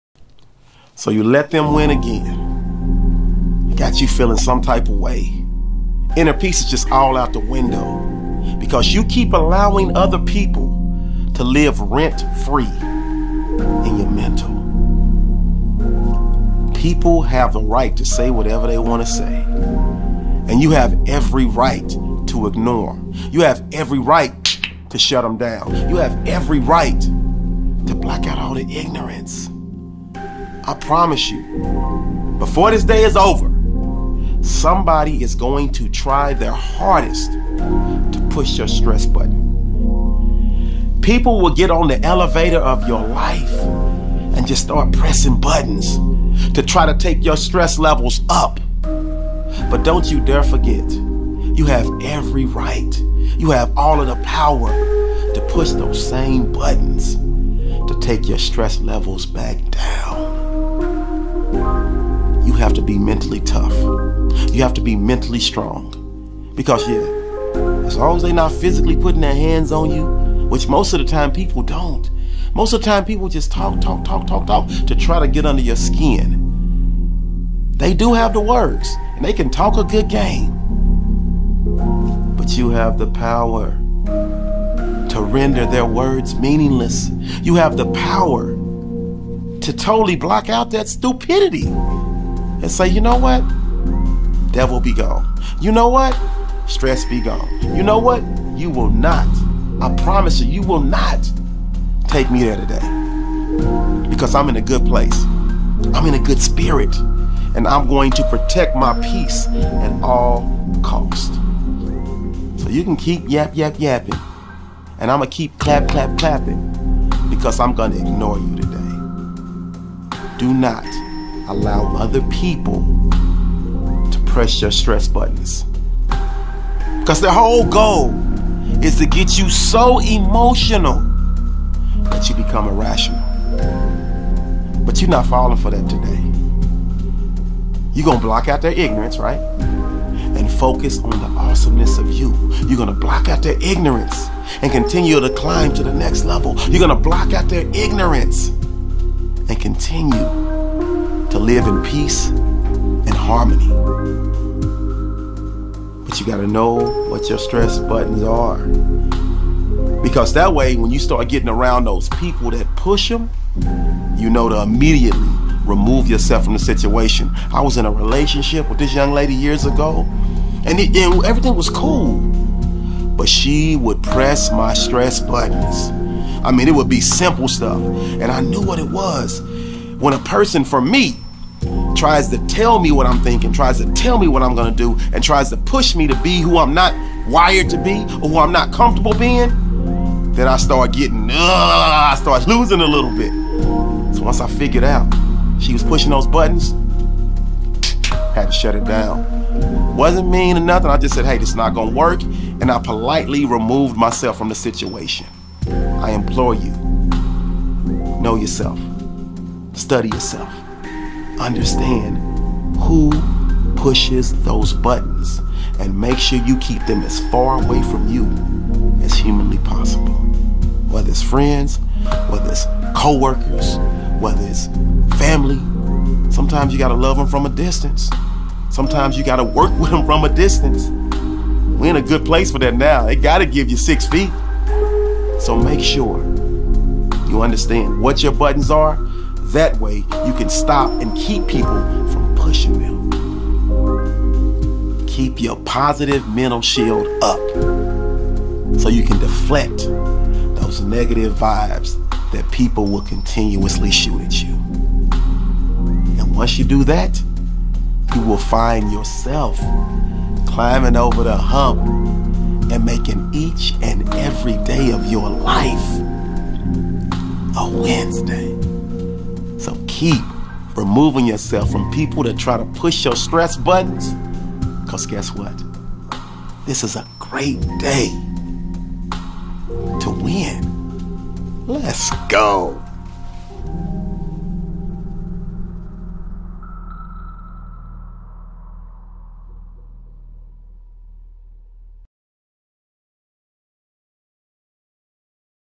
Don't Let Them Press Your Stress Buttons | Daily Motivation Dose | Freddy Fri
I made this video so that you don't let them press your stress buttons, stop letting them live rent-free in your mind!  My name is Freddy Fri and I am here to teach you how!

This episode is designed to help you stop people from pressing your stress buttons, many of us do it, we let them win again so that you end up feeling some type of way, throwing that inner peace right out the window!

Stop allowing other people to live rent-free in your mind, don't allow them to stress you out!

No, I am not saying people should have the right to say whatever they want to say, because they do, but on that same note you also have every right to ignore them, you have every right to shut them down, you have every right to block out all the ignorance!

You and I both know it before the day is over somebody is going to try their hardest to push your stress button.  People will go on the elevator of your life, and just start pressing buttons to try to take your stress levels up, but don't you dare forget that you have every right, to push all those buttons to take your stress level down!

It is true you have to be mentally tough, you have to be mentally strong because as long as people are not physically putting their hands on you, you still have the power within to take their words and render them meaningless, you have the power to totally out that stupidity and just say devil be gone!